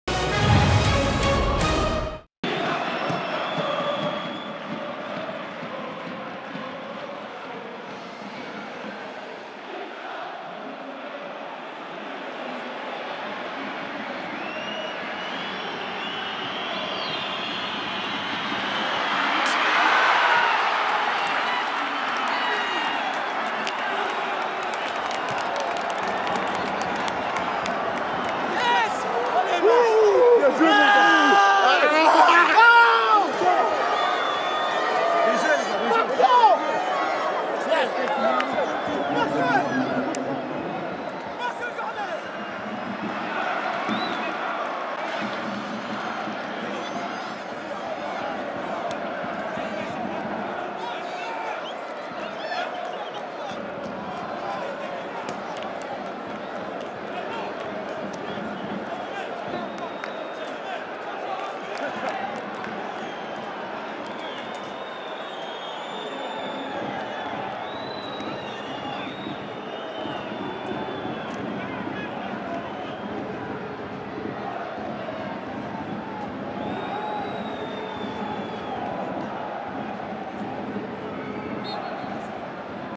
But Maxwel CORNET (20') / OGC Nice - Olympique Lyonnais (0-5) / 2017-18
OGC Nice - Olympique Lyonnais (0-5) - But de Maxwel CORNET (20'). Tous les buts de OGC Nice - Olympique Lyonnais en vidéo.

Allianz Riviera - dimanche 26 novembre 2017

Buteurs : Memphis DEPAY (5' - Olympique Lyonnais) / Maxwel CORNET (20' - Olympique Lyonnais) / Mariano DIAZ (27' - Olympique Lyonnais) / Memphis DEPAY (38' - Olympique Lyonnais) / Myziane MAOLIDA (79' - Olympique Lyonnais)